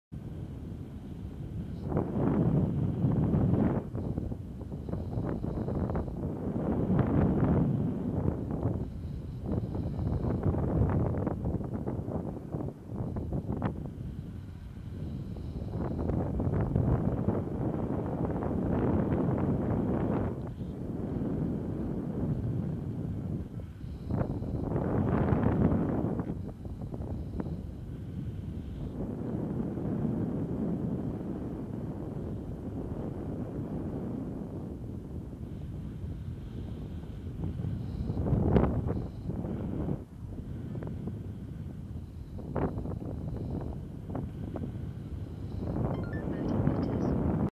Paragliding - Mt Woodside, Canada - Gin Carrera Plus - MOHOC 1080p60 - 2016-10-22 - GliderHUD
Testing out GliderHUD.